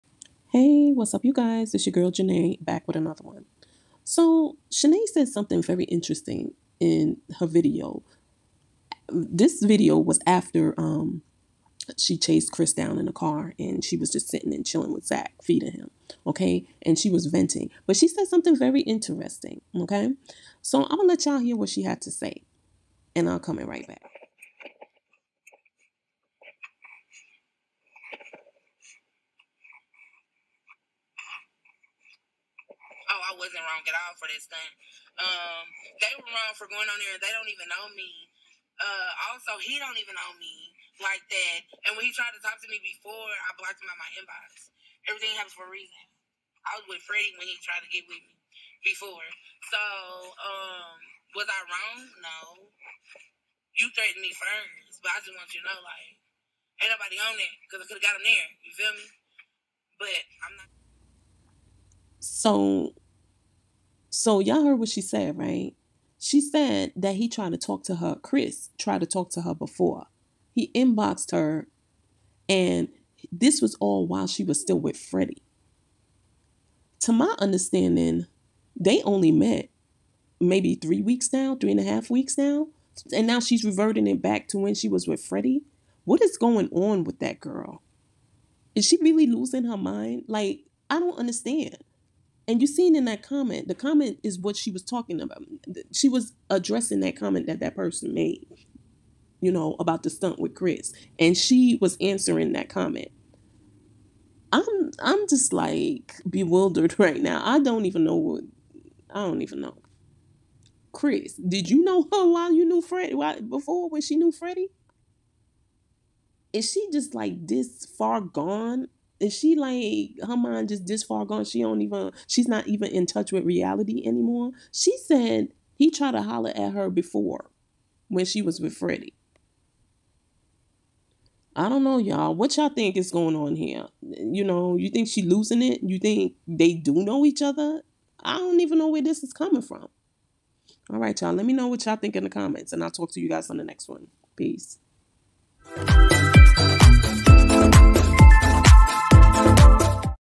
Schenee, Chris did what...
Schenee says that Chris is the one that tried to holla at her first.